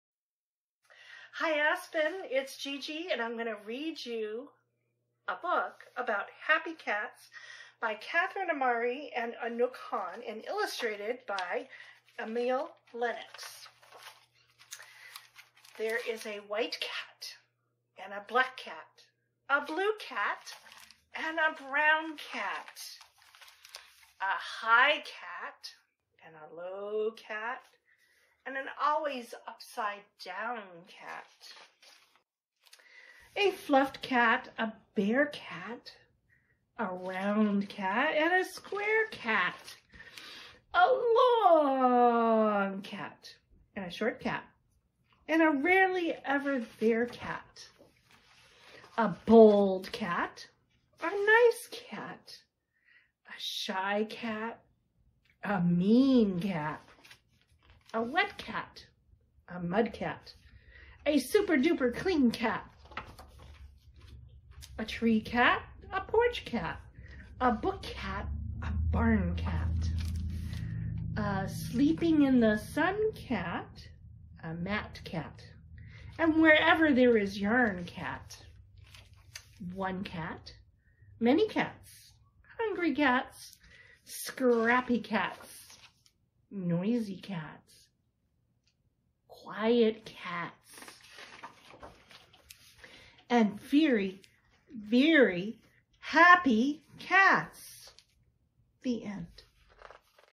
Gigi - Happy Cats
A childrens story by Catherine Amari and Anouk Han, illustrated by Emi Lenox.   The story is read by GiGi for Aspen.